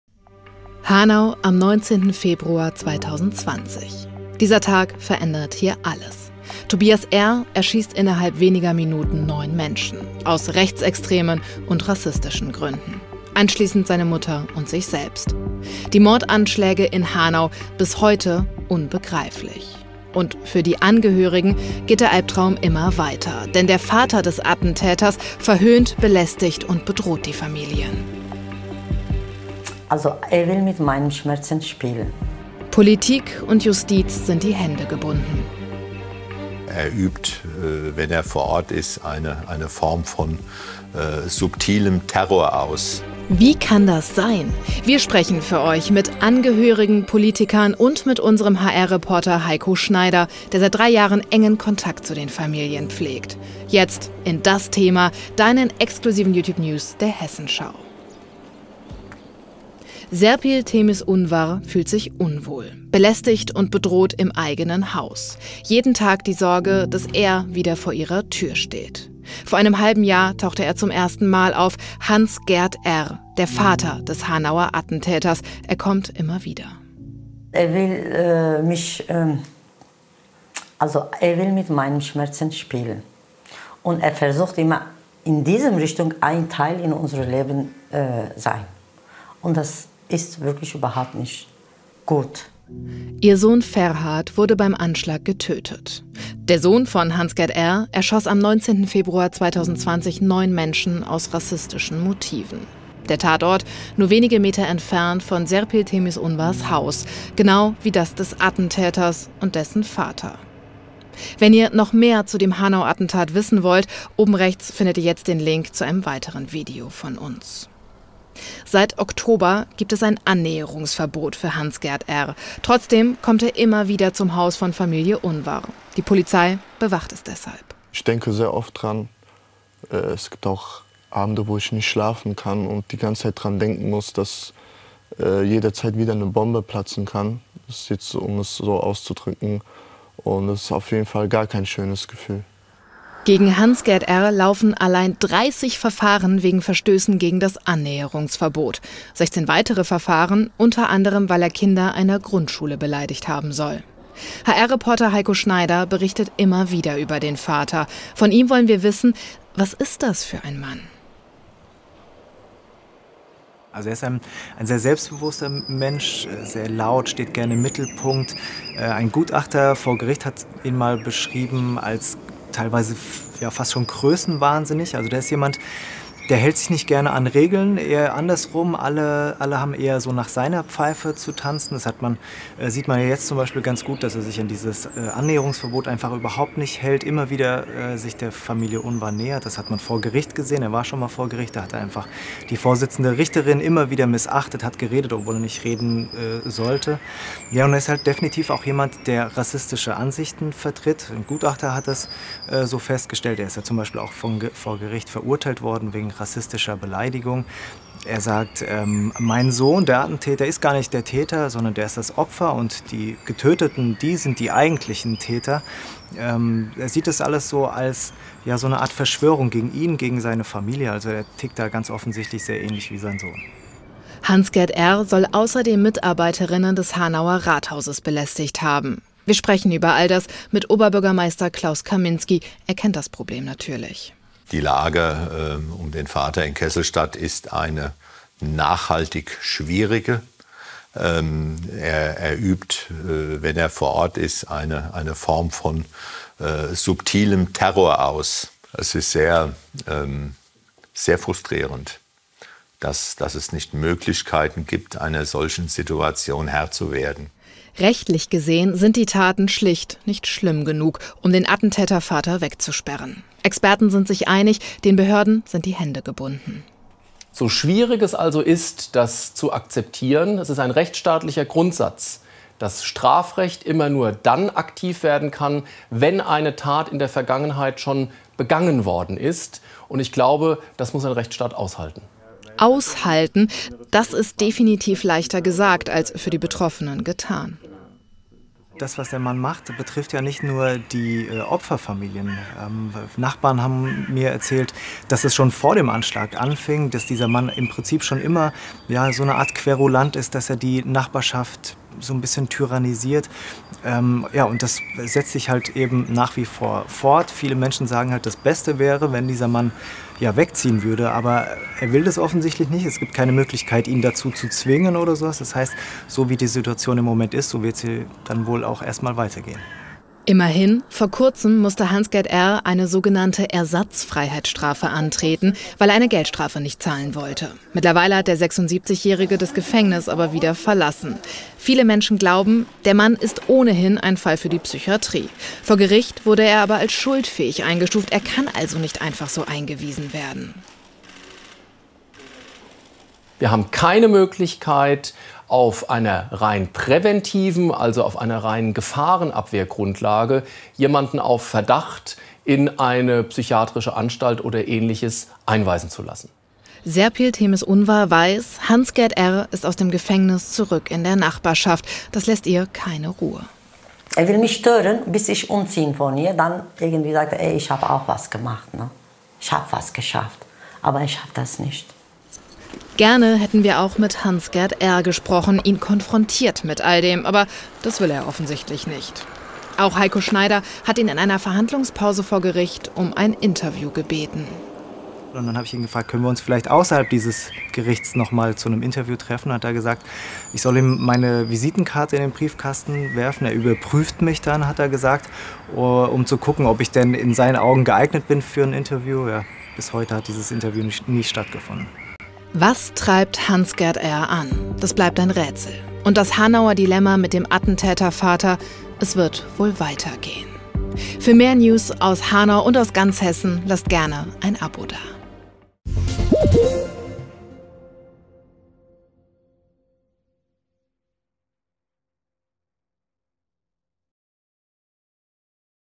Vater des Hanau-Attentäters terrorisiert Opfer-Familien. Wie kann das sein? I hessenschau DAS THEMA
„Subtiler Terror“ – das beschreibt ganz gut, was derzeit die Überlebenden und Hinterbliebenen der Opfer des rechtsterroristischen Anschlags von Hanau durchleben müssen. Denn der Vater des Attentäters belästigt und bedroht die Familien. Auch mehr als drei Jahren nach der schrecklichen Tat kommen die Angehörigen nicht zur Ruhe. Für sie geht der Albtraum immer weiter.

Heute bei DAS THEMA, deinen exklusiven YouTube News aus Hessen, bist du dabei, wenn wir mit Serpil Temiz Unvar reden, der Mutter des getöteten Ferhat Unvar. Über den Vater des Attentäters sagt sie: „Er will mit meinen Schmerzen spielen.“ Aber eins sagt sie auch, nämlich dass er es nicht schaffen werde. Außerdem wollen wir wissen, wie es eigentlich sein kann, dass der Vater des Attentäters die Familien bis heute noch terrorisiert.

sowie täglich um 19.30 Uhr im hr-fernsehen und im Anschluss hier auf YouTube.